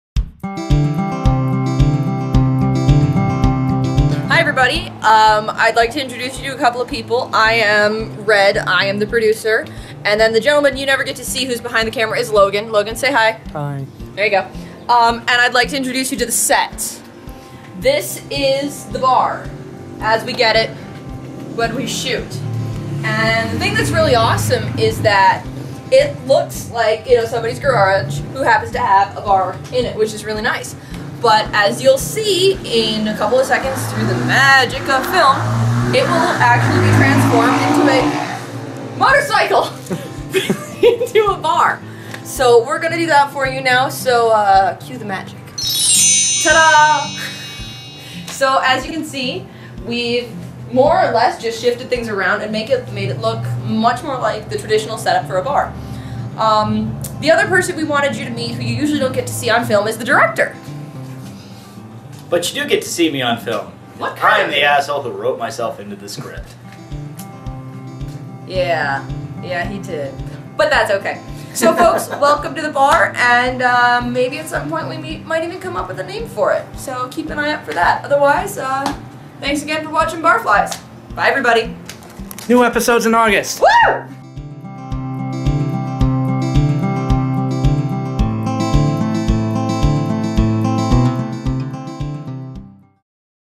Bar Flies- Behind the Scenes- Set Tour
Our lovely and amazing producer for Bar Flies shows you our new garage set.